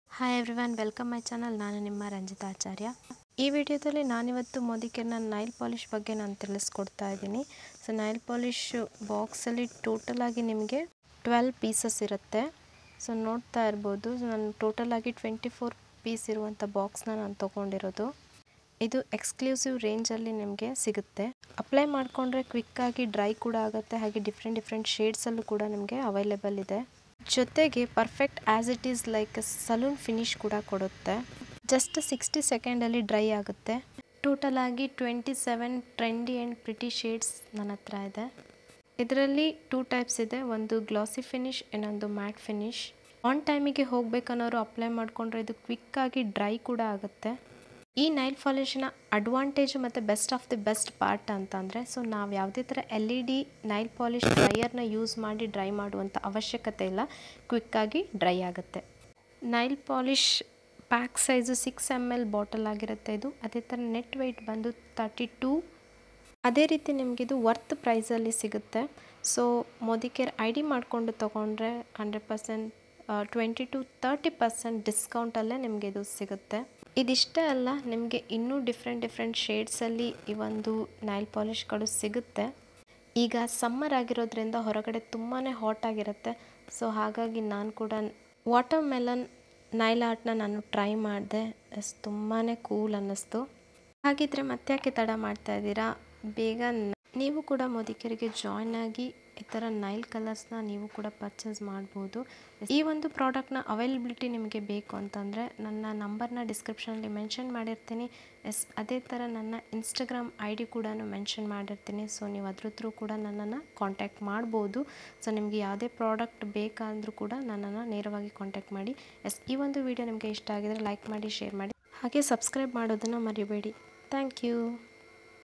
Modicare Urban Color Landon Nail Lacquer || Nail art with 27 different Shades 💅🏻💅🏻- ಕನ್ನಡ
Modicare Urban Color Landon Nail Lacquer

Urban Color Matte Nail Lacquer features 12 trendy colors with a matte finish . These highly pigmented colors are shine-free and appear opaque on application giving a smooth , flawless, long lasting color to nails . The formulation has excellent drying time along with being 3 -free, non-yellowing & chip-resistant.

Features & Benefits:-

Shine –free, matte finish
Highly pigmented shades deliver a strong color to nails
Flawless nails ; hides nail imperfections easily
Application is smooth & streak free
Toulene, DBP and Formaldehyde Free